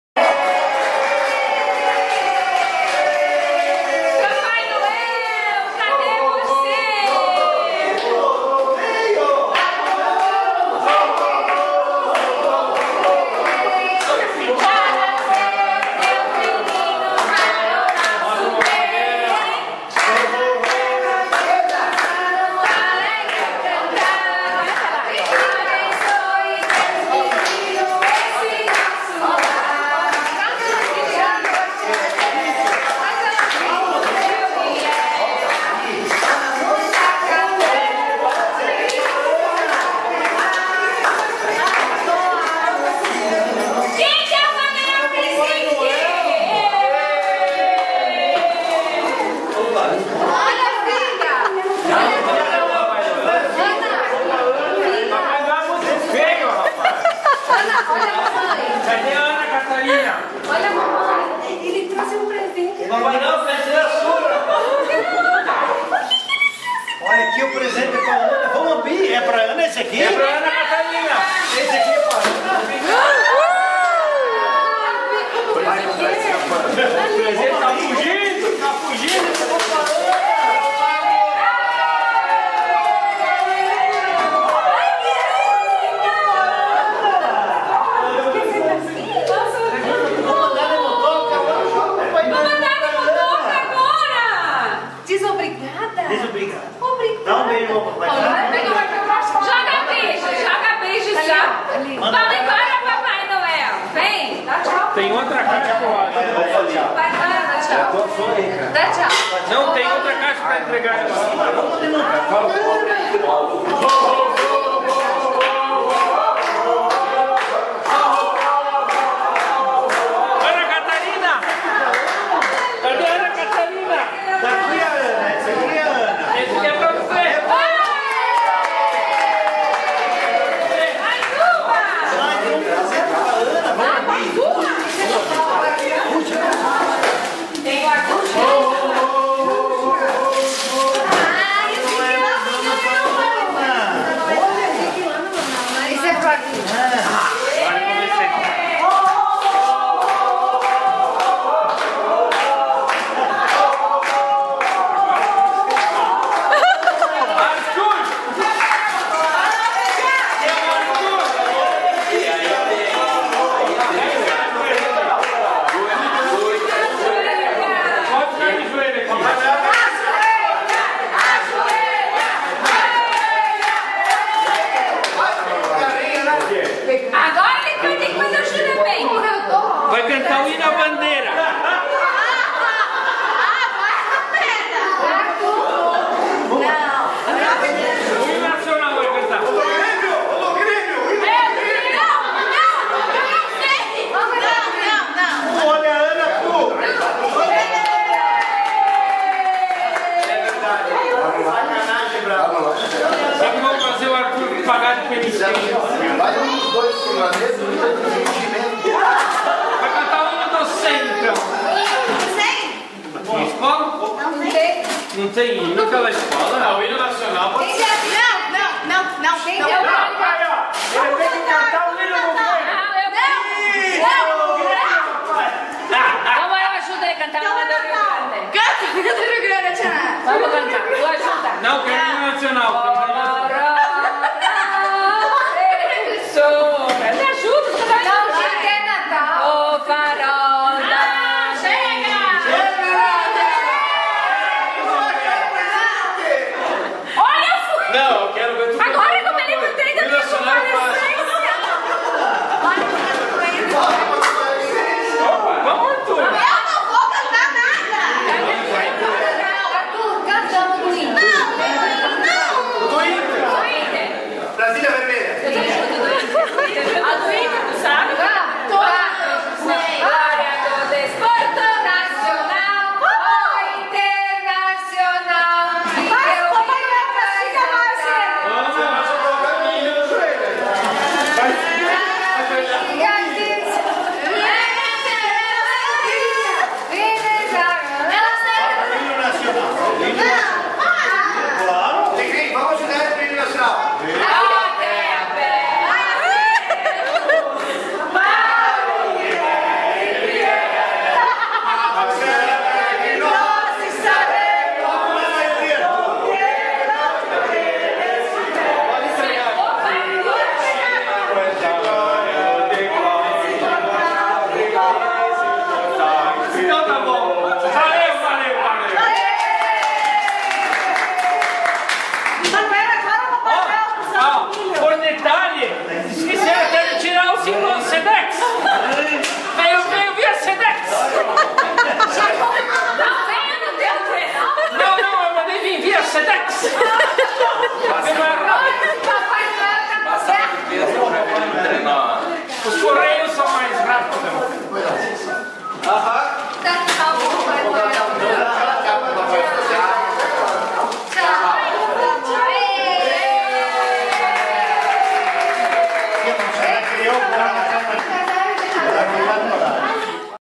Natal da família Cordazzo em Seara - Chegada do Papai Noel
Momento em que o Paulinho Noel entregou os presentes da Anna Catharina e do Arthur. Participações especiais: Lili, Negrin, Nádia, Paulo, Nilza, Serginho, Gili, Lu, Glau, Jô, Diogo, Dori, Fiorello, Minga, Inês e Brunelli.  Cinegrafista (amadora): Ciça.